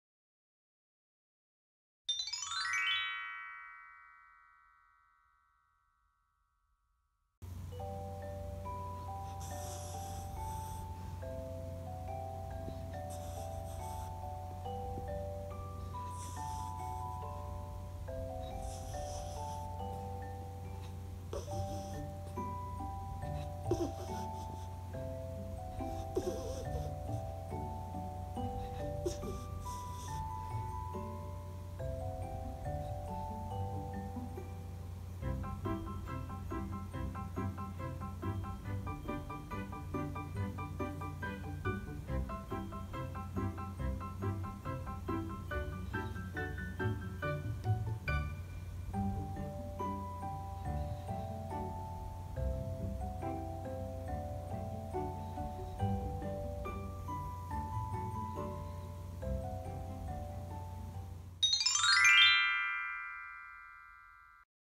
[뚱숑별이TV]똥배여신(?) 별이의 달콤한 낮잠타임 코로로로옹 /비숑프리제//강아지낮잠//강아지잠꼬대/
얘두라~ 우린 꿈에서조차 열일을 해쟈나 구치!
나 지곰 달콤한 단잠에 빠져있지만 꿈속에서 열찌미 푸르른 초원을
뛰뛰 해능중이야 히^^